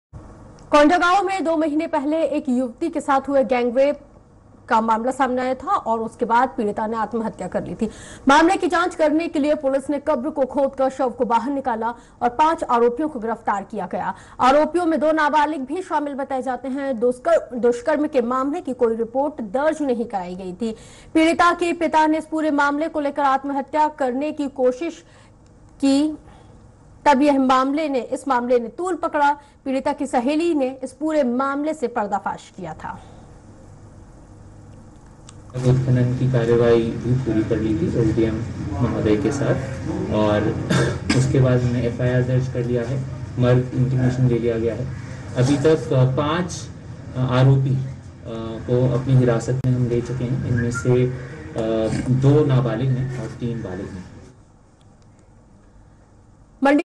Chhattisagrh Gangrape News | नाबालिग लड़की के साथ 7 लोगों ने किया गैंगरेप…
Chhattisagrh Gangrape News | नाबालिग लड़की के साथ 7 लोगों ने किया गैंगरेप….पीड़िता के आत्महत्या के बाद भी पुलिस ने दर्ज नहीं किया केस….अब कब्र खोदकर निकाला शव |